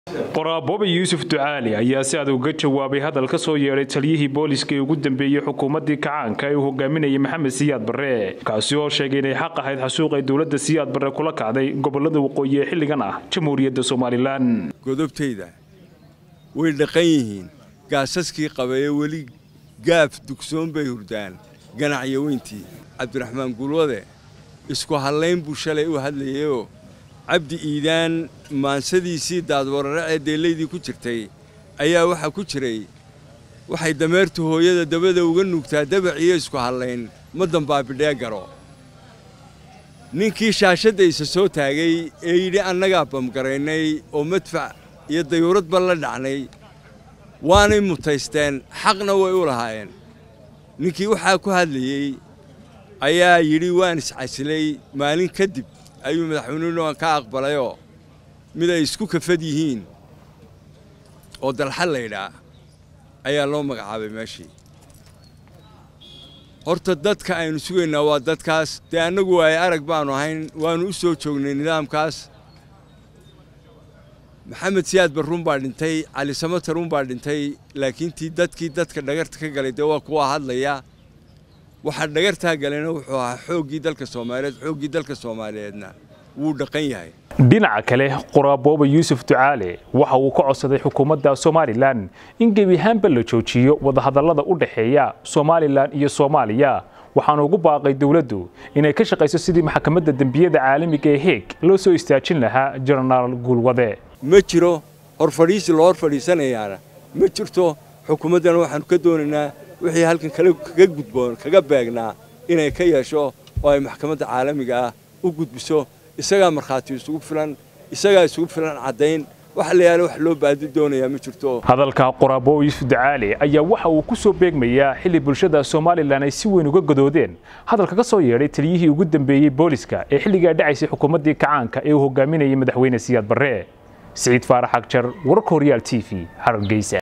Somaliland oo Maxkamad Caalamiya la Tiigsanaysa General Guul-wade oo Taageeray Xasuuqii Siyaad Bare
Somaliland oo Maxkamad Caalamiya la Tiigsanaysa General Guud Wade oo Taageeray Xasuuqii Dawladii Siyaad Bare